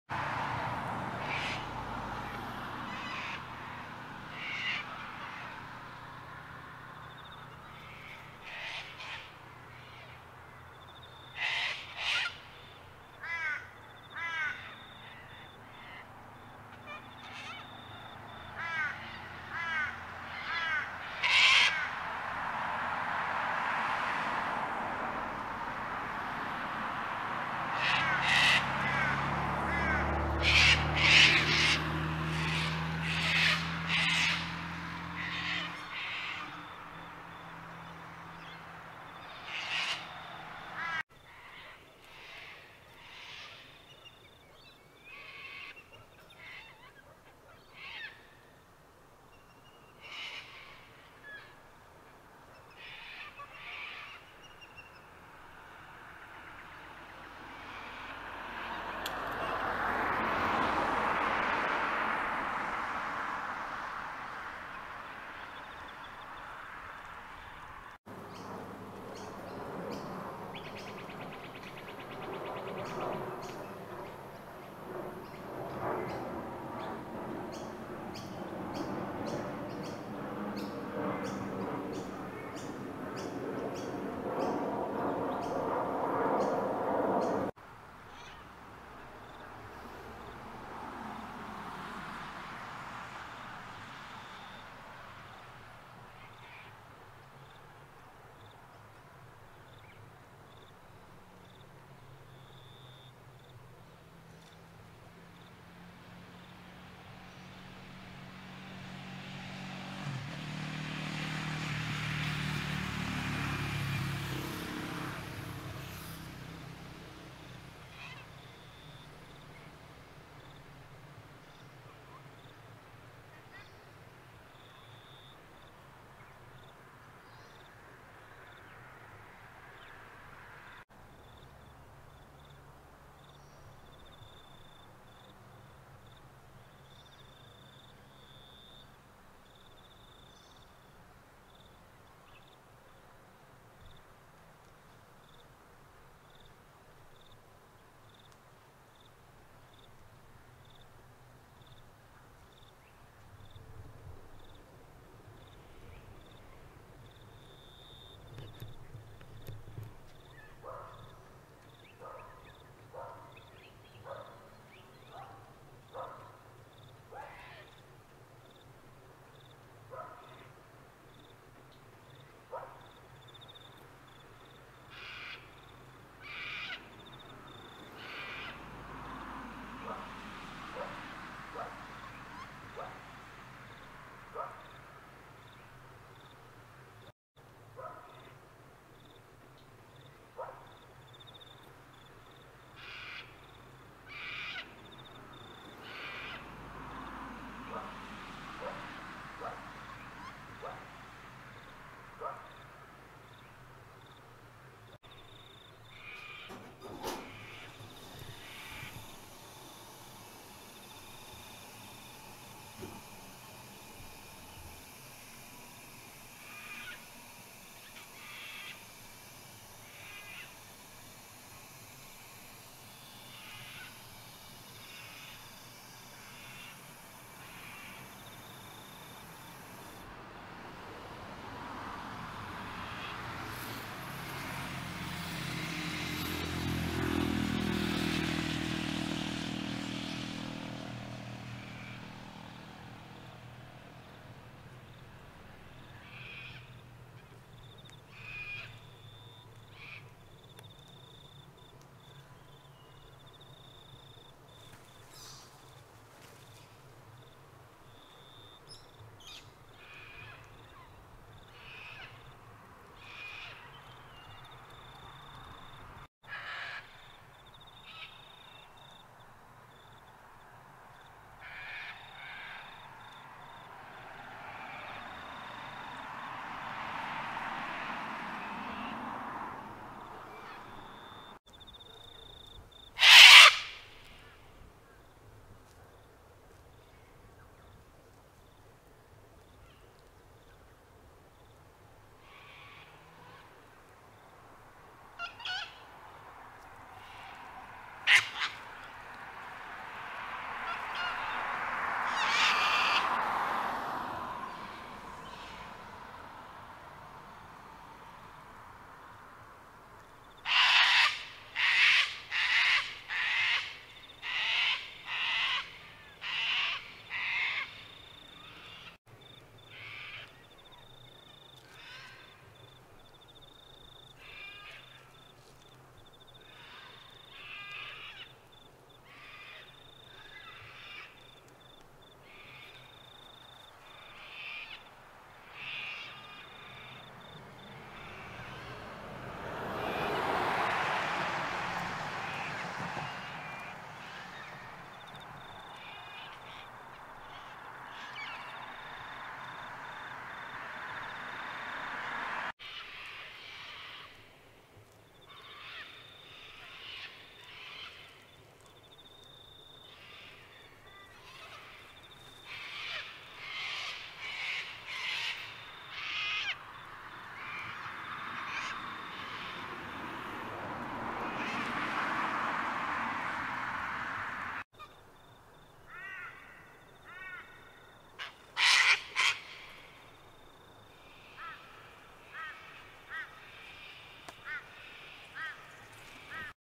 Sulphur-Crested Cockatoos at Mt Coot-tha, Nov 2022
Sulphur-Crested Cockatoos at Mt Coot-tha in Brisbane, Queensland, recorded with Canon R7 on Nov 6, 2022.